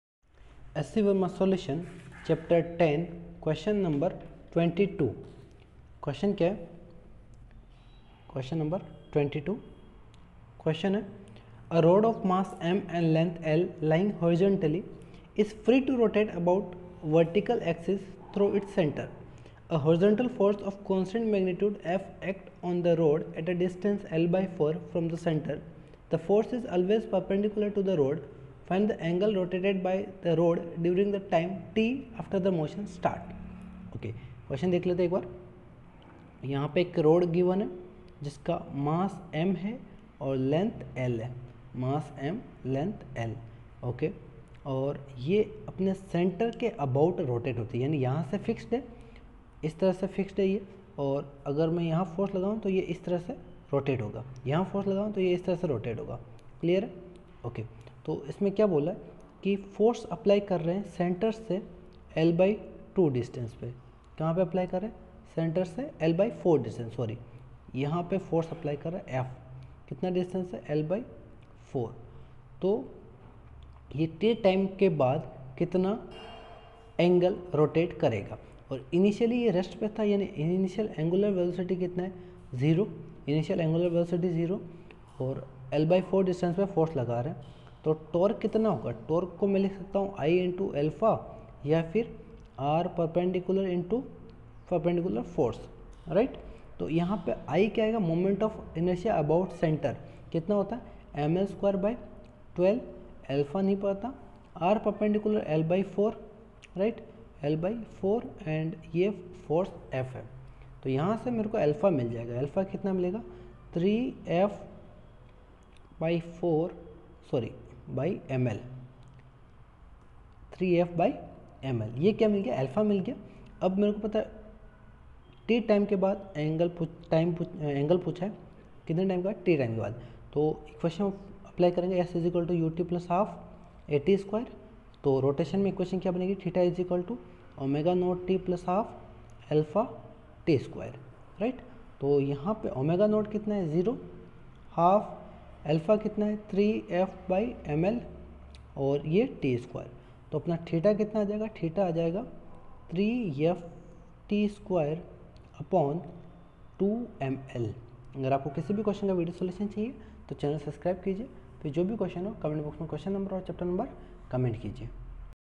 H.C. Verma Solutions - Rotational Mechanics - Chapter 10 , Question 22
22. A rod of mass m and length L, lying horizontally, is free
to rotate about a vertical axis through its centre. A
horizontal force of constant magnitude F acts on the rod
at a distance of L/4 from the centre. The force is always
perpendicular to the rod. Find the angle rotated by the
rod during the time t after the motion starts.